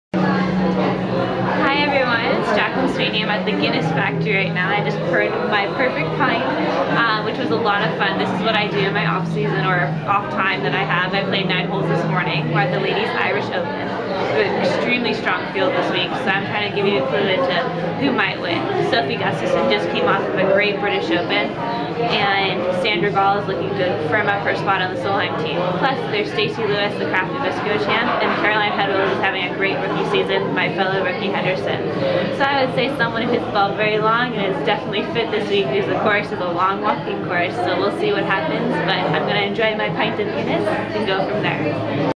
Jaclyn Sweeney blogs from the Ladies Irish Open
Henderson Rookie Jaclyn Sweeney video blogs from the Ladies Irish Open